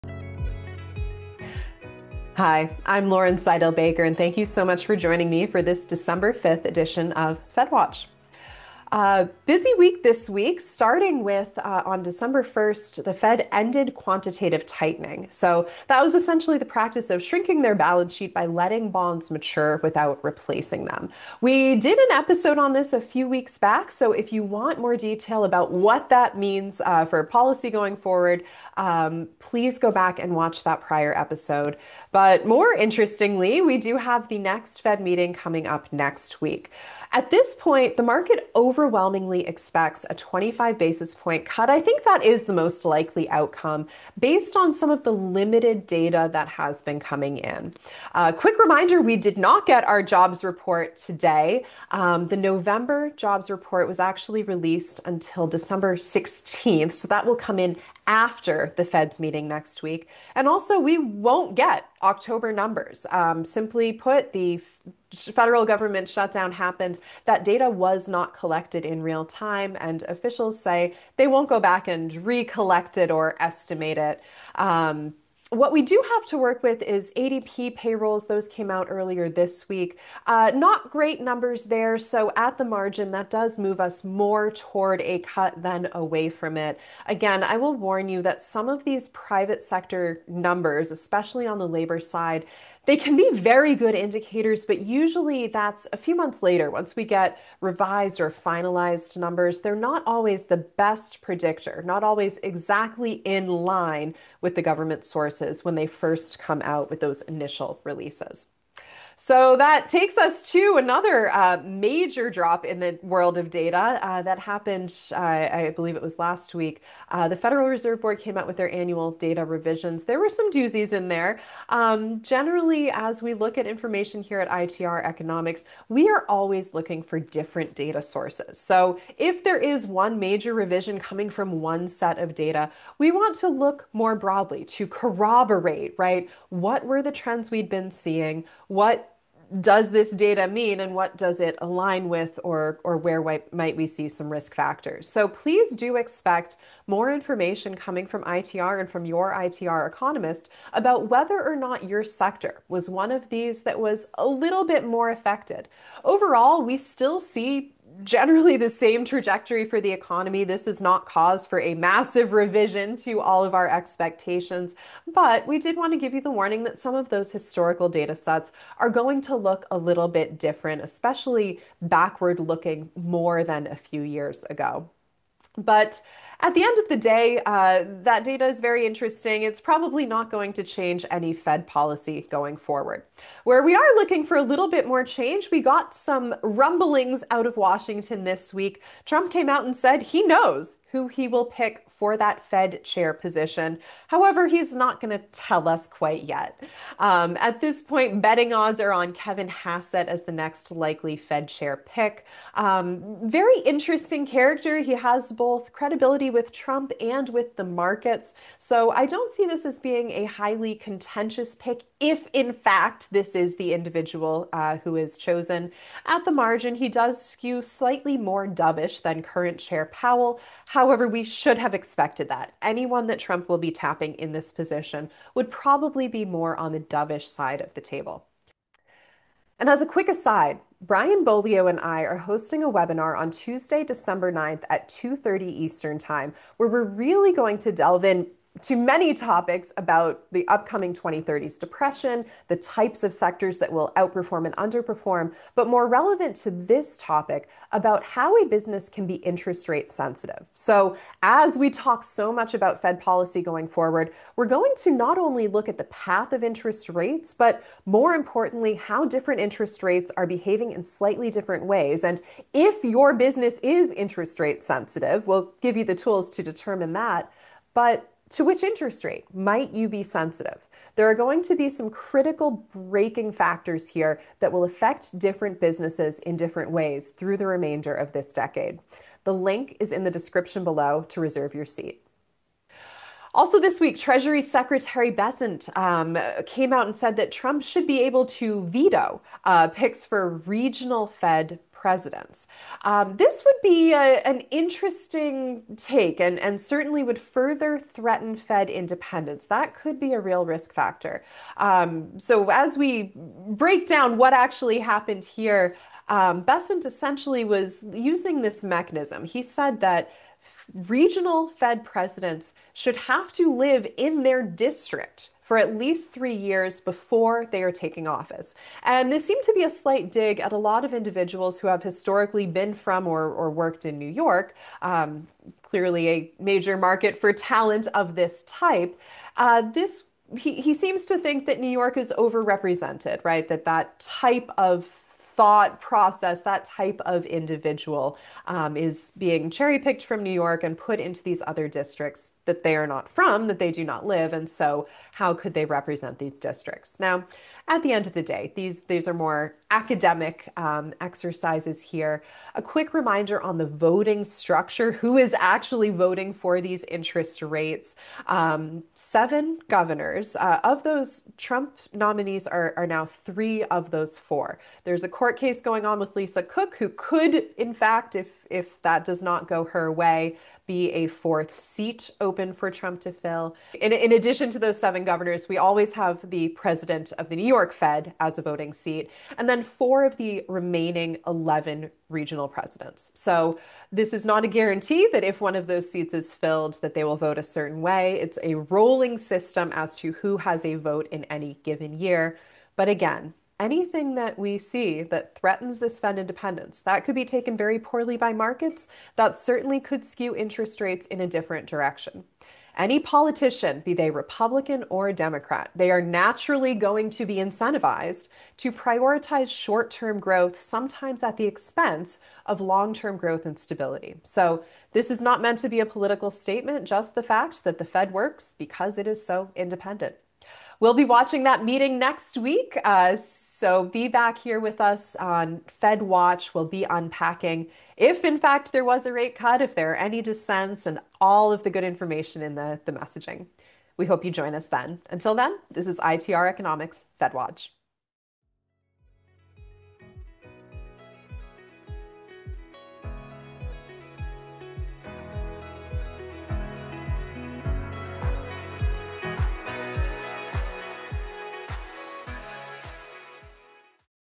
Will the End of Quantitative Tightening BOOST the Economy? | Fed Watch
This week on Fed Watch, ITR Economist and Speaker Lauren Saidel-Baker breaks down a pivotal moment for monetary policy as markets price in a likely rate cut ahead of next week’s meeting. With government data delayed by the shutdown, businesses are left navigating uncertainty and relying on less-predictive private-sector indicators. Lauren also highlights major Federal Reserve data revisions that could reshape how leaders interpret historical trends, plus new political developments that may influence future Fed leadership and independence.

If your business is interest rate sensitive, this episode offers timely context to help you prepare for rate-driven risks and opportunities. What change in policy would impact you most?
Join us for our upcoming webinar to discover the essential tools and strategies you will need